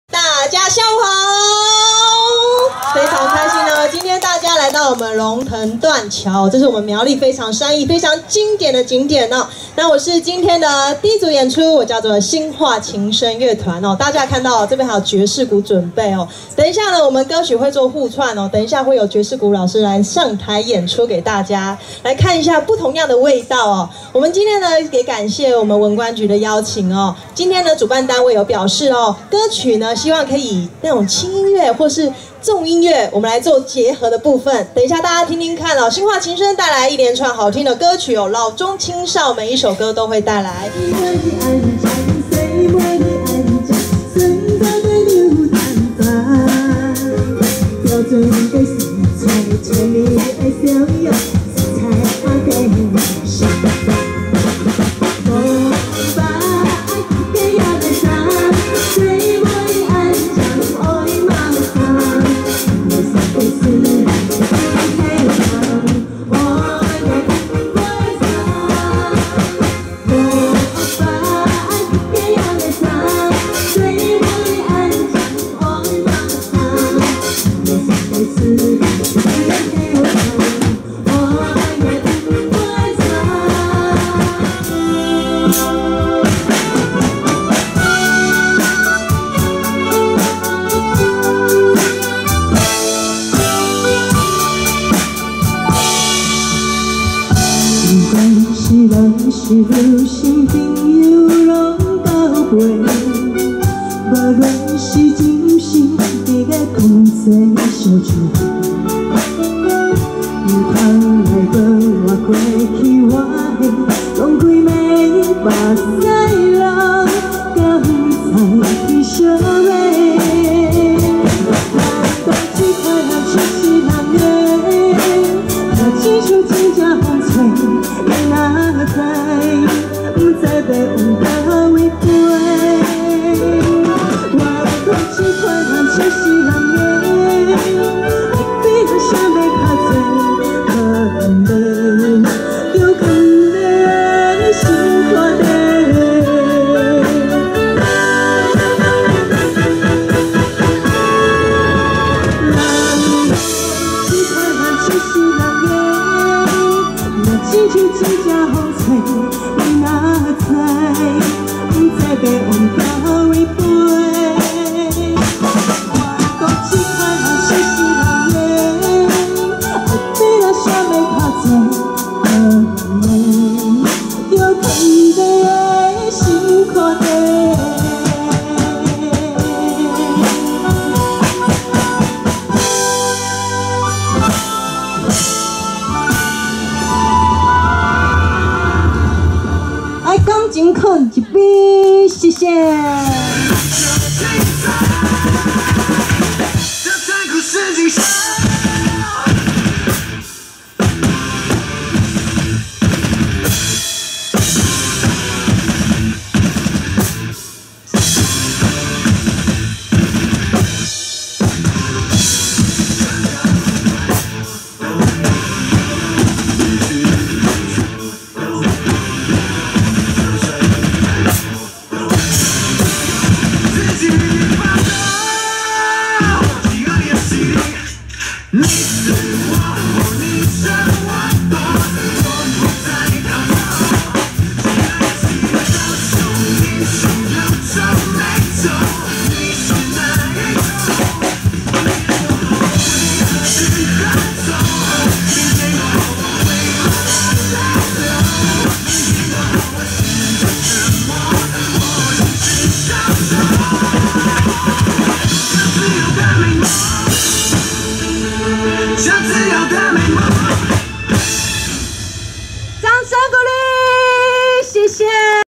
2017三義鐵道文化節
這次表演用電子琴彈唱+爵士鼓的結合一起演出唷!!
現場貴賓回應相當熱情唷!!
不一樣的音樂演出 就找"欣樺琴聲音樂工作室"吧!!~^^~
FB粉絲專頁: